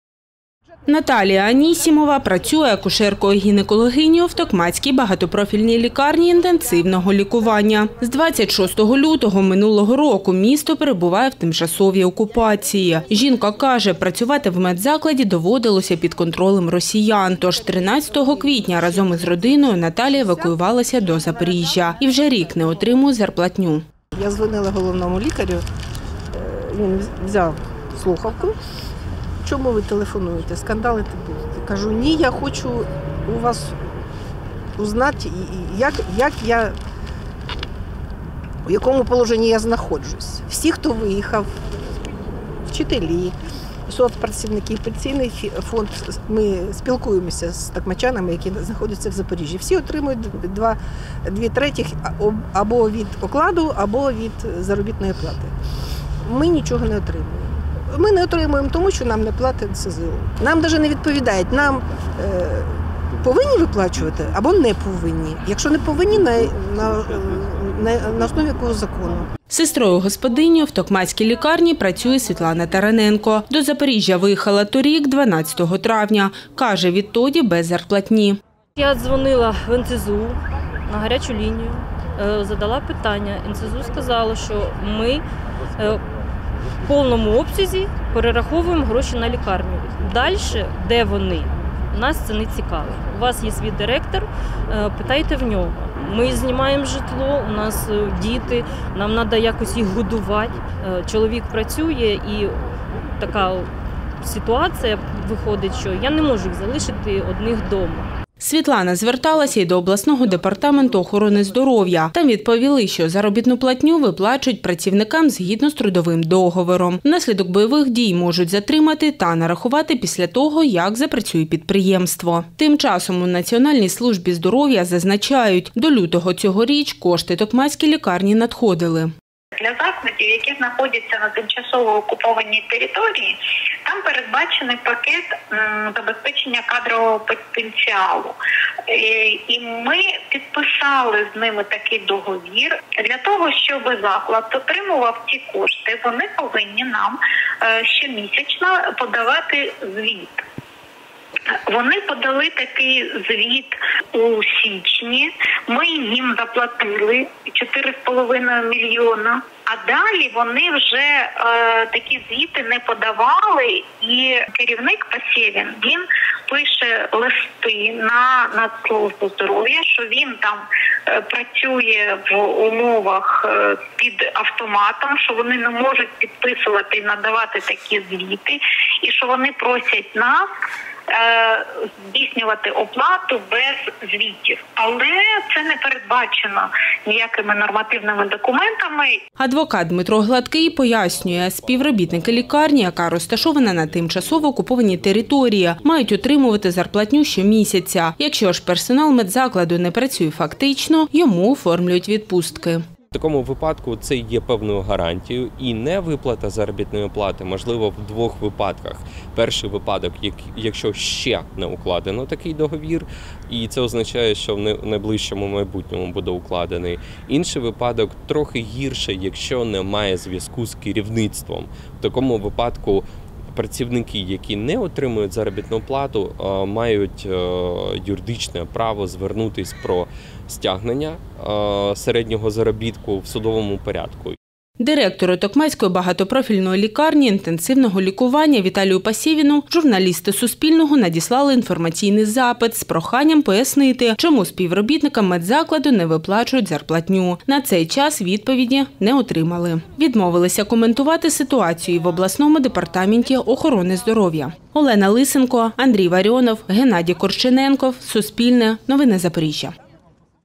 Медикам Токмацької лікарні не платять заробітну платню. В чому причина? | Новини
Медичним працівникам Токмацької багатопрофільної лікарні інтенсивного лікування, які виїхали з тимчасово окупованого міста, майже рік не платять заробітну платню. Чому так сталося –  розбиралися журналісти Суспільного.